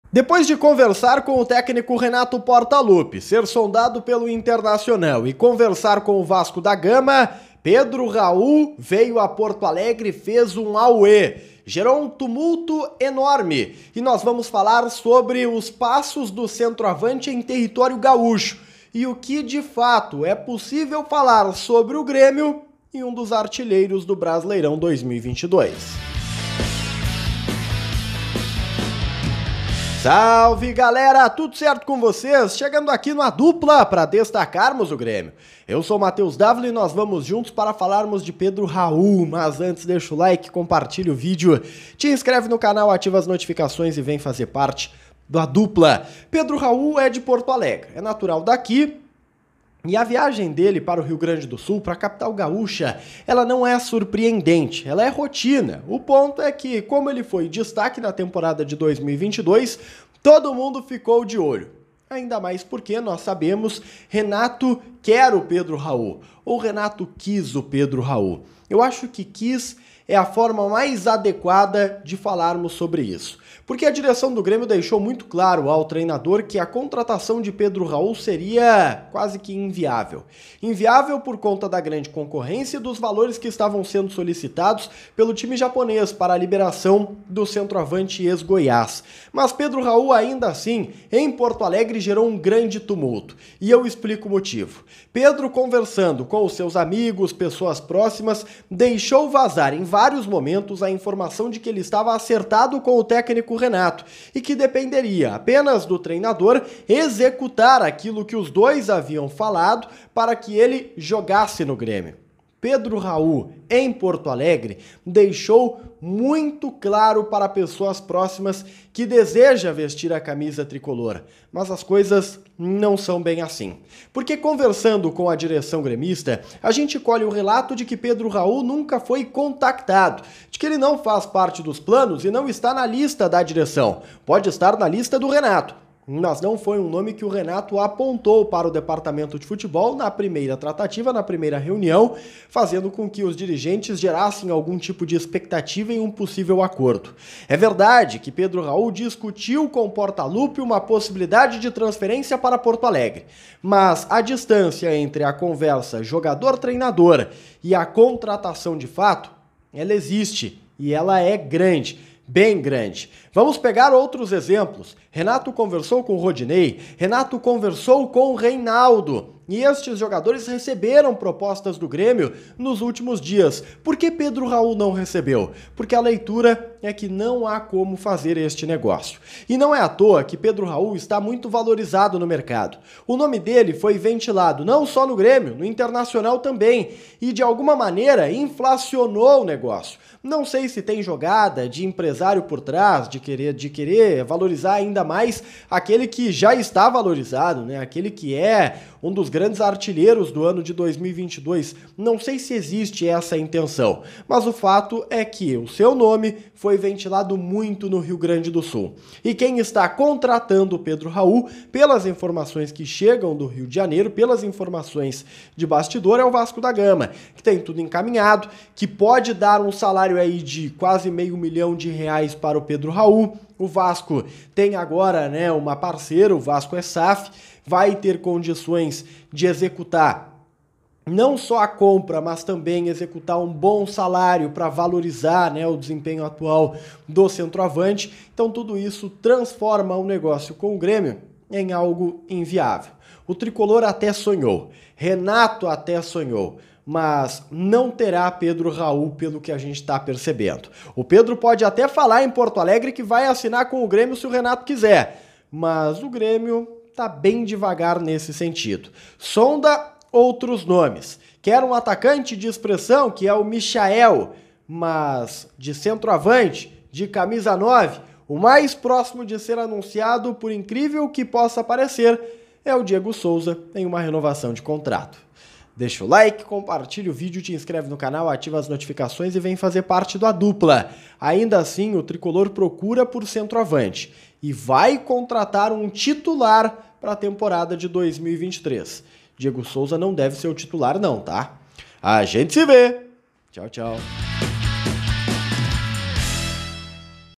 🔵⚫ Grêmio: O que Pedro Raul disse em Porto Alegre | Vontade manifestada | Conversa com Renato
Use o cupom A DUPLA lá na KTO e ganhe 20% em cima do seu primeiro depósito.

PORTO ALEGRE É PEQUENA, PEDRO...

VASCO

Segue o D'Avila no instagram: @davilamatheus